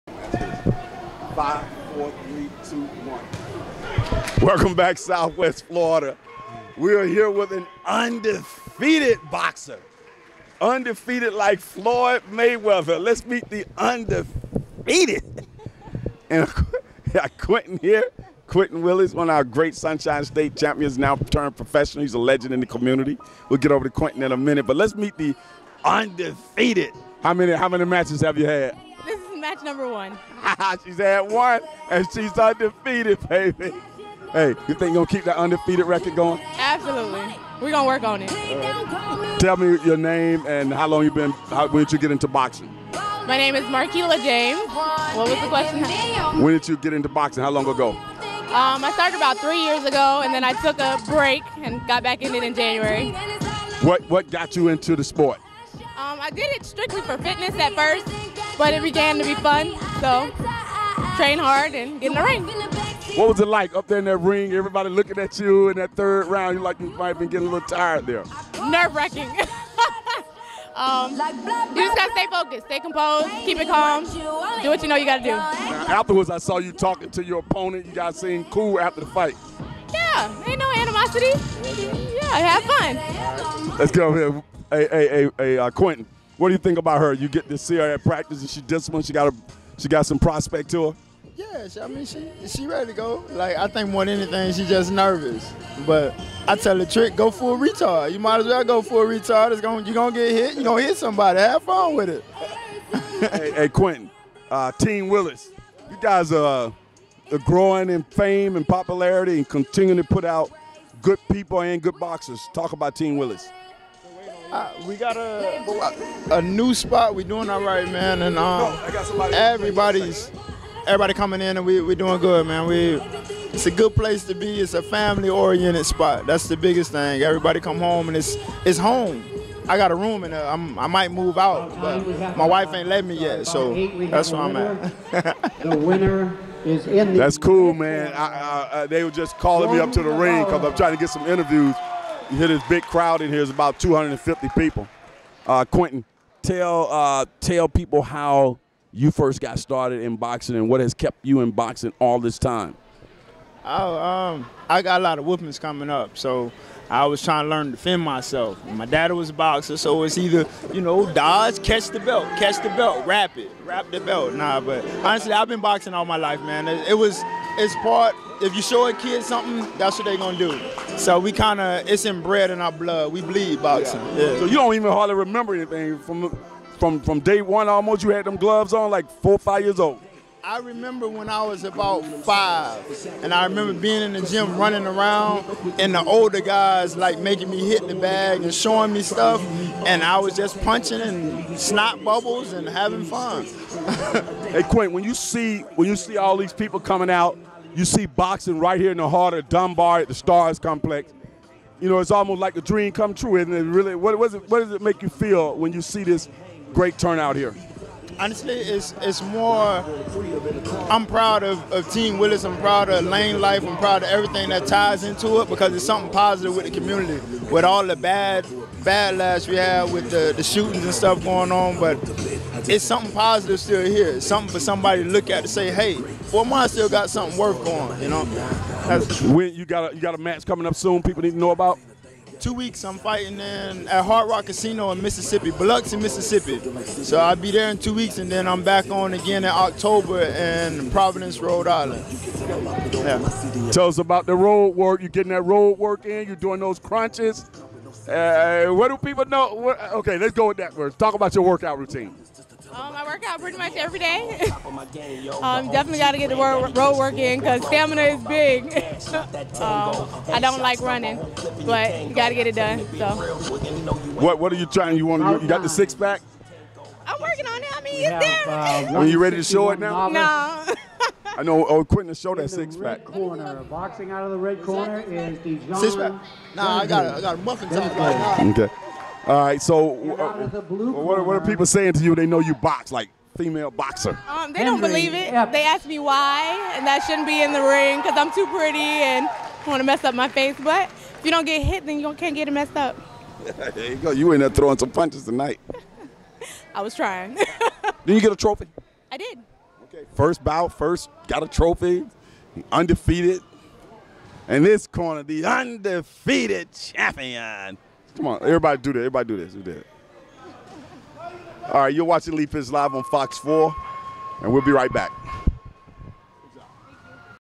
Marquilla James and Quenton Willis, boxers, chat with Lee Pitts Live on FOX 4
Glover Not Guns 2. Boxing